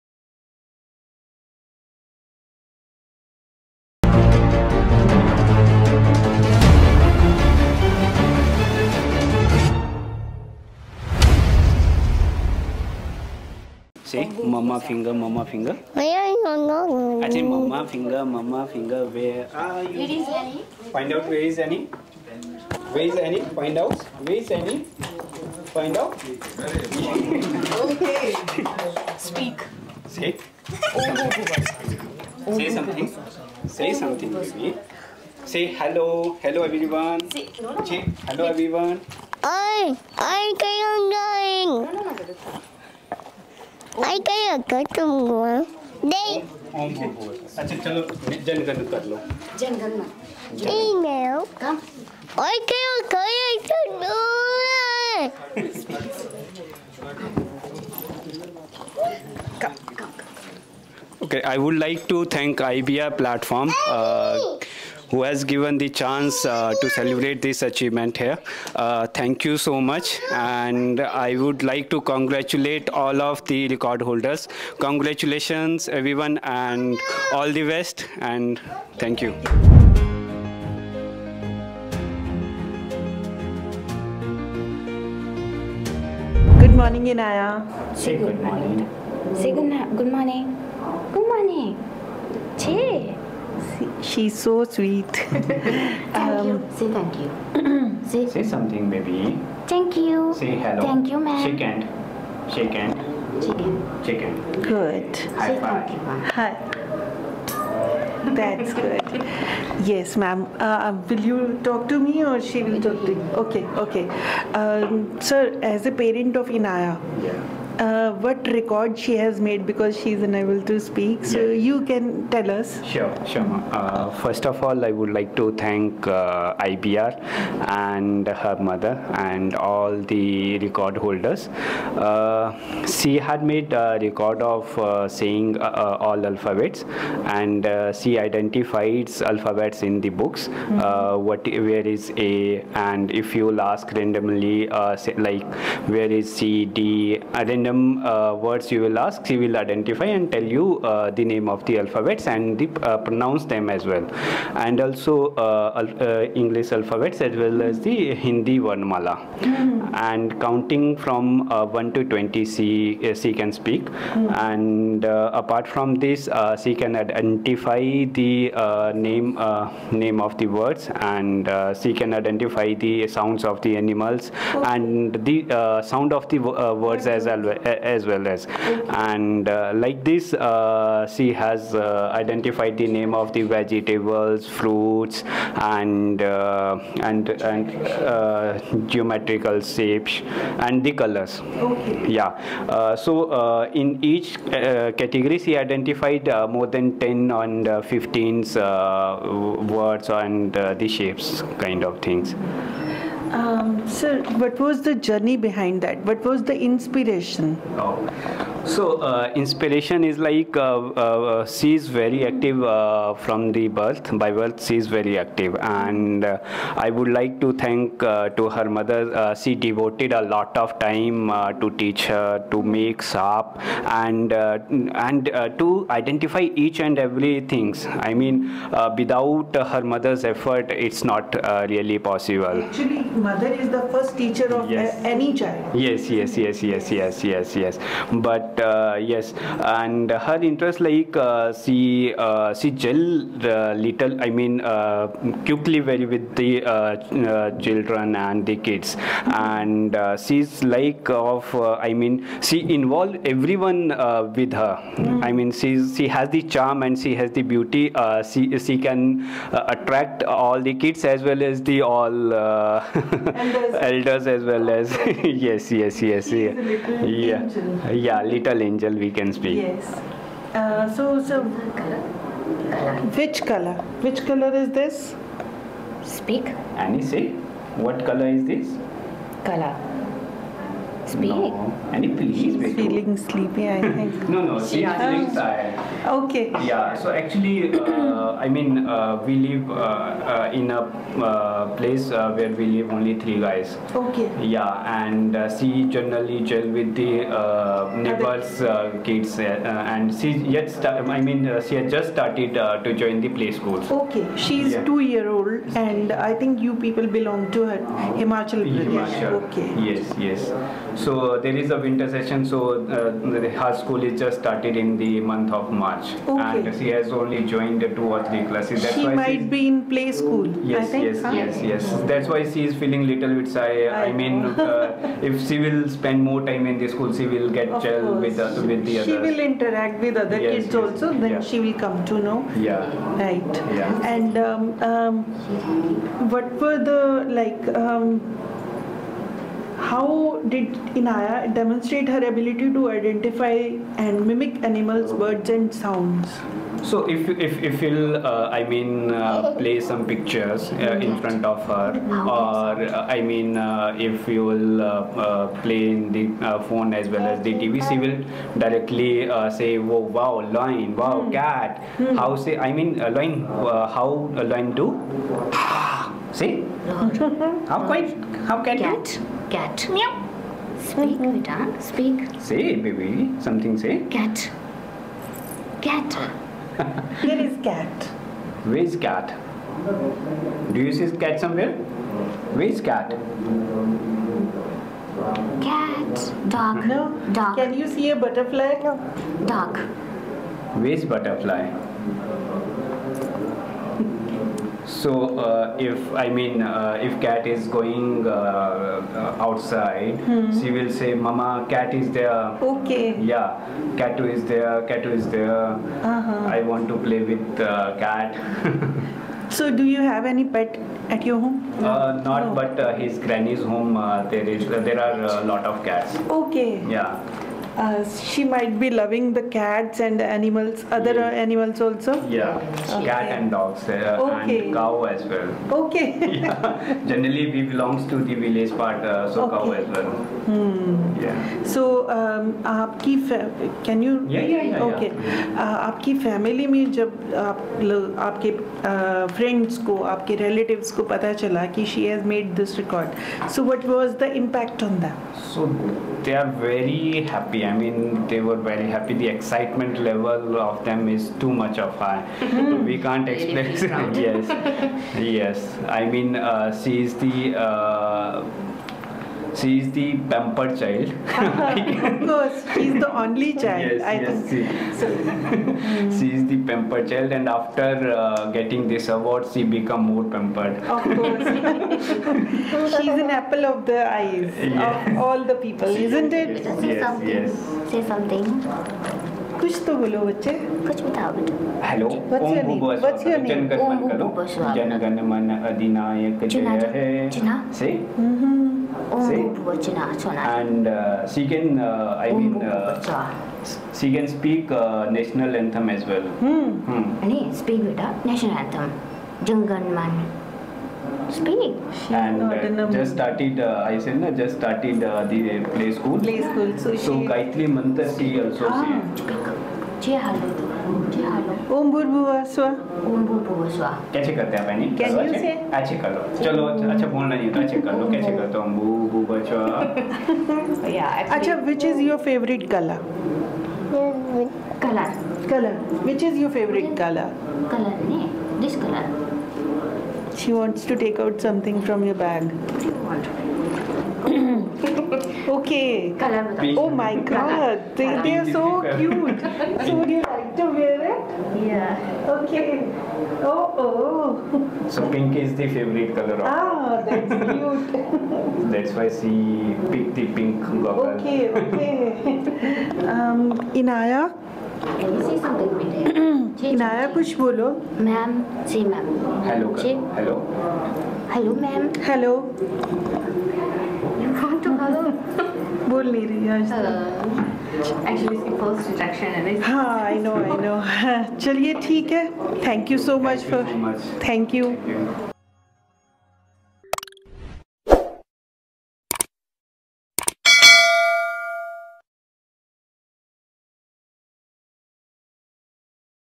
Tiny Talent: Enaaya Thakur's Remarkable Achievements
Enaaya Thakur, from Mandi, Himachal Pradesh, has captured hearts and accolades with her remarkable abilities. At the tender age of just 2 years and 4 months, she's been honoured as an 'IBR Achiever' for her impressive skills. Enaaya can identify 16 animals, 12 birds, 6 colours, 5 shapes, 7 cartoon characters, and 12 parts of the body. She also recognizes 44 letters of the Hindi Varnamala and associated words of the English alphabet. Enaaya's talent extends to mimicking the sounds of 7 animals, showcasing a depth of understanding and creativity beyond her years. Her achievements serve as an inspiration, highlighting the limitless potential of young minds.